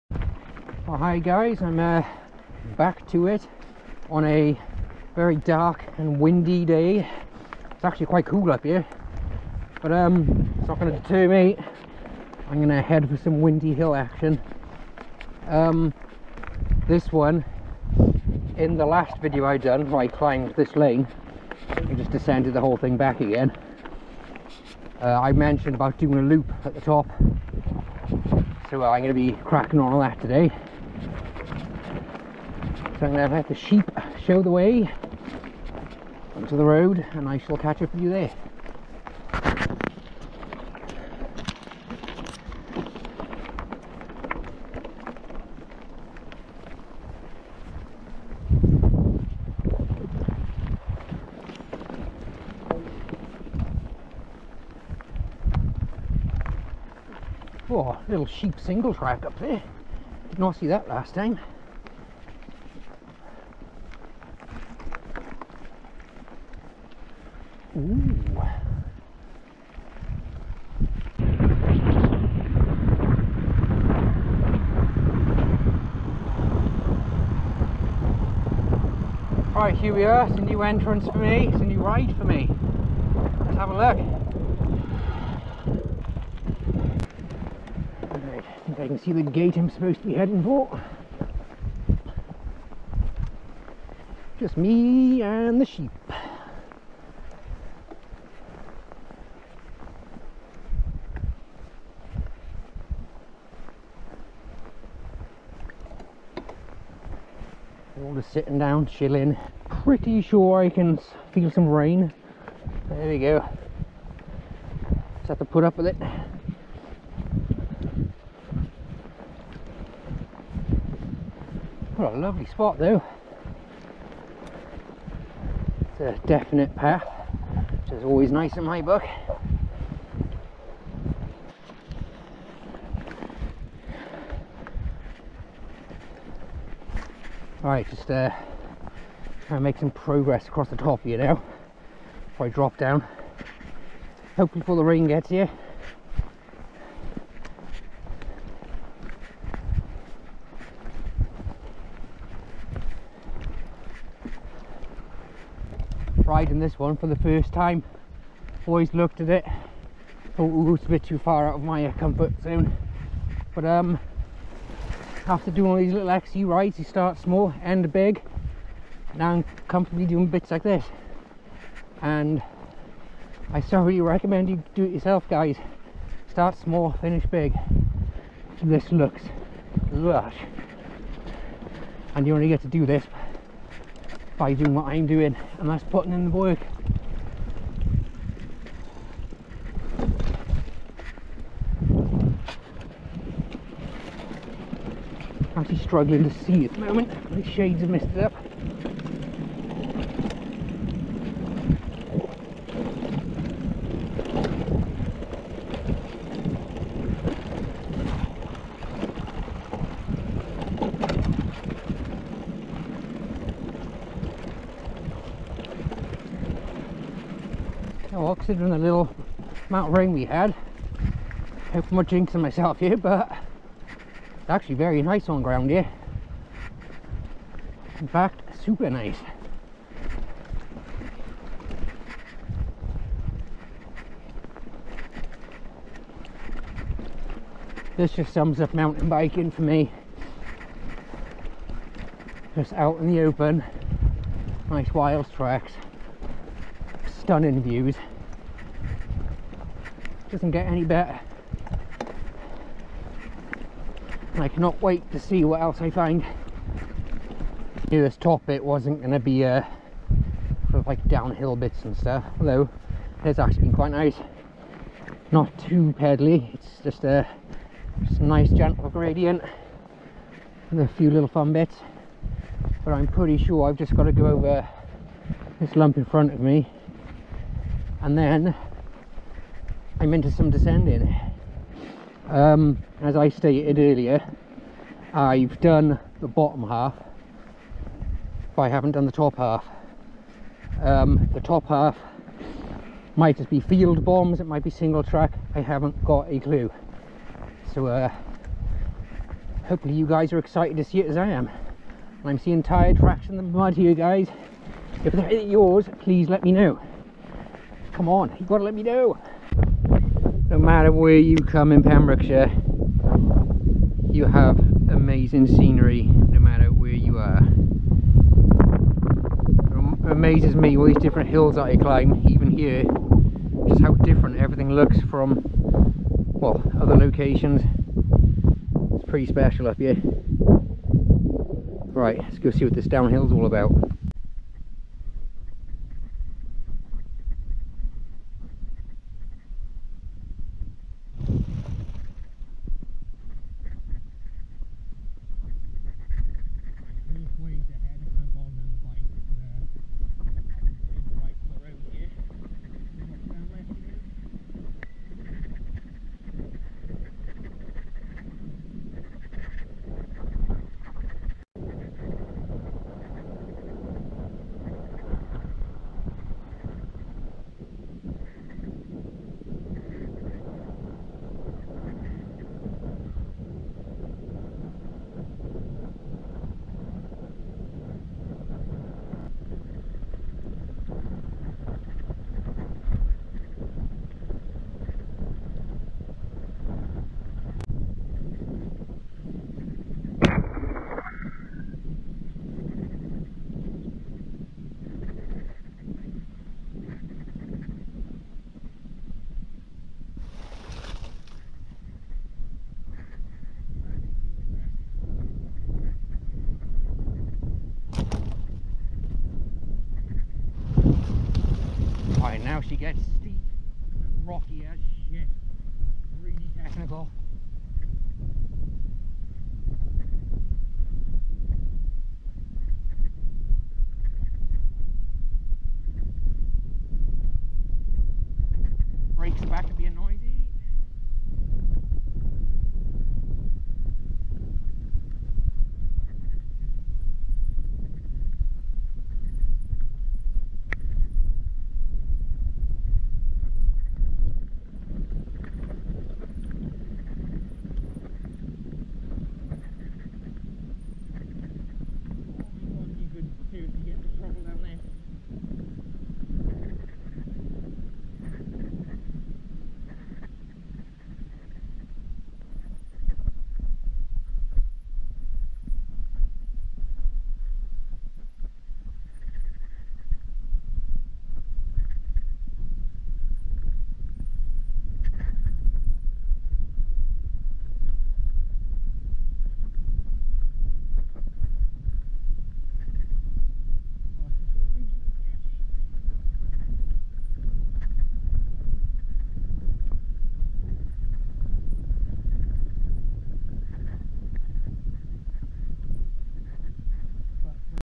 A day on the hills. Gwaun valley loop 2
I hope you like hill climbs because theres plenty on this one!!! the bebefits though are amazing views and a long (but easy) descent back to the bottom. Once at the gate at the end of the video simply turn right and follow the road back 2 miles to get to the car park.